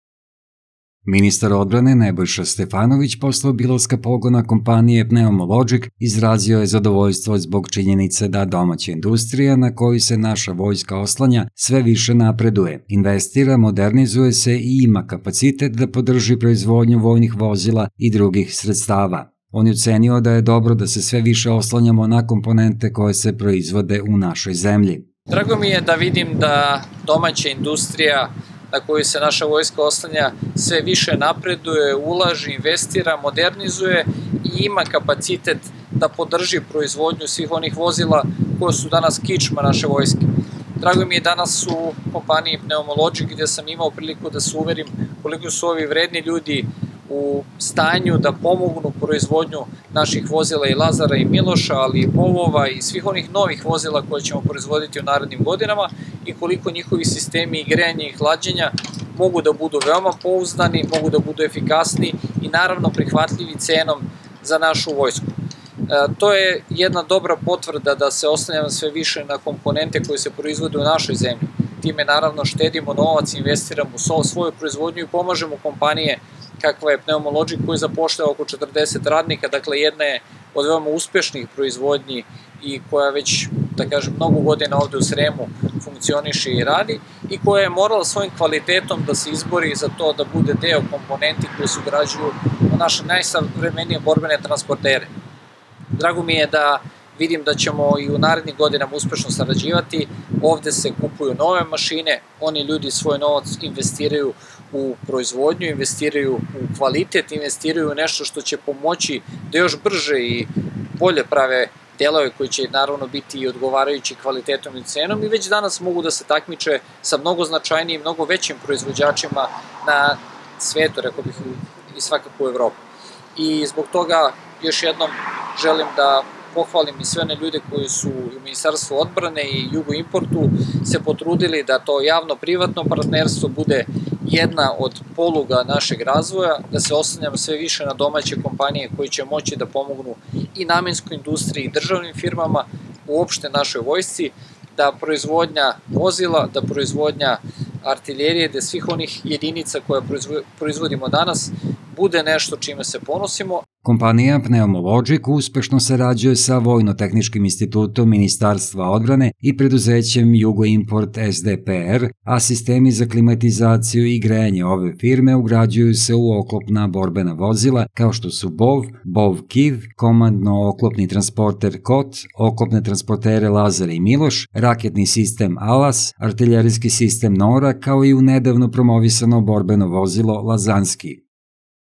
Nebojša Stefanović - Poseta kompaniji Pneumo Lodžik
Potpredsednik Vlade i ministar odbrane Nebojša Stefanović posetio je danas kompaniju „Pneumo Lodžik“ koja se bavi proizvodnjom sistema za klimatizaciju i grejanje borbenih vozila iz proizvodnog programa srpske odbrambene industrije.